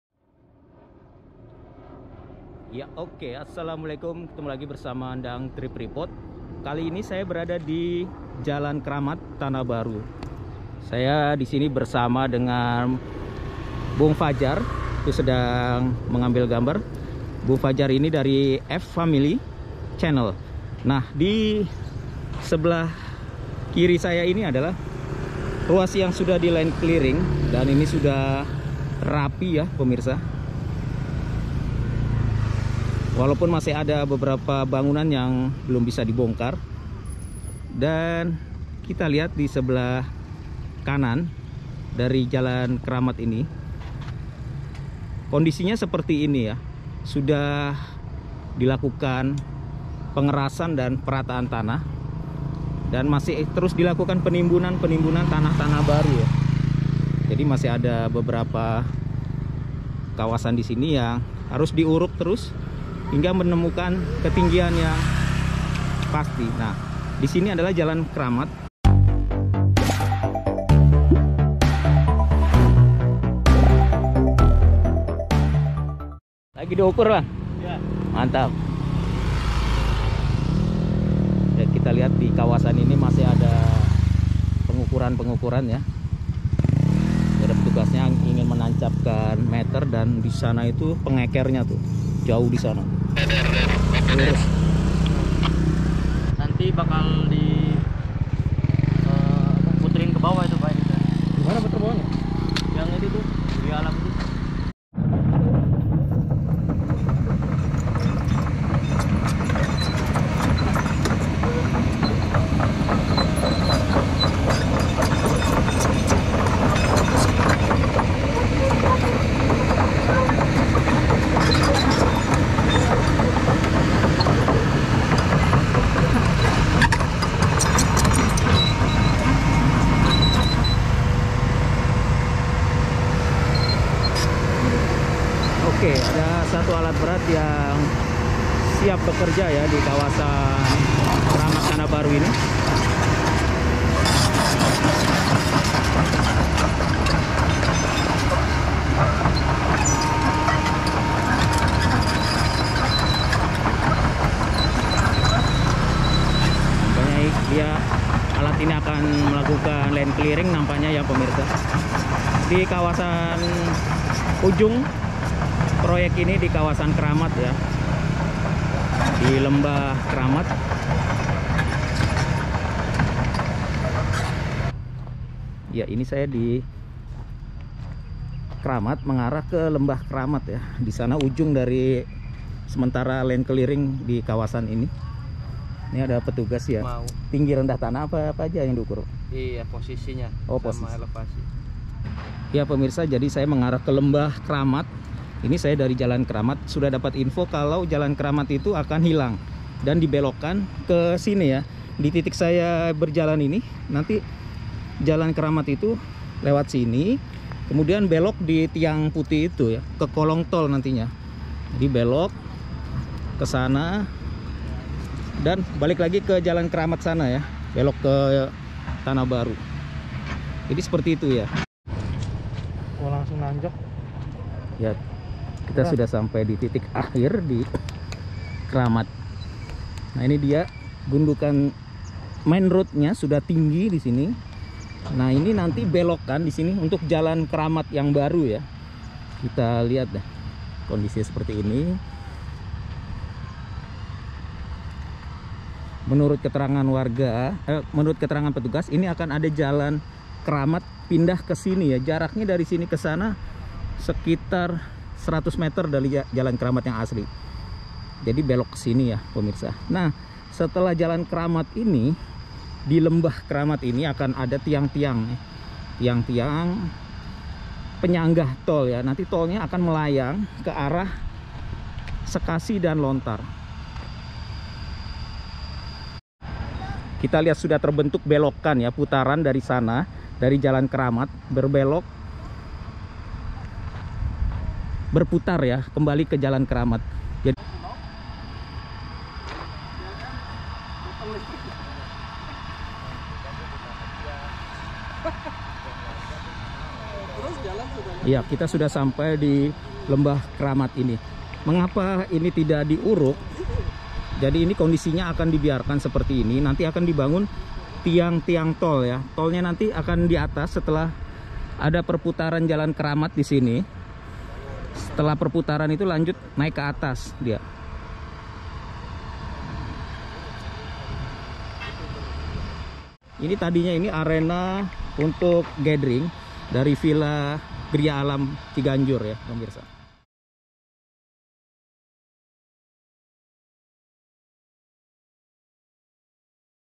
Jalan Kramat Tanah Baru Bergeser Ke Kolong Tol Dekat Lembah
Jalan Kramat Tanah Baru terdampak tol cijago seksi 3 dibelokkan ke arah lembah kemudian akan berputar di kolong tol.

Kamera : GoPro Hero 9, Samsung A50s (video 2020),
Gimbal : Brica SteadyPro untuk HP
Edit : VN & Adobe Premiere Pro CC

Untuk bisnis dan informasi hub call center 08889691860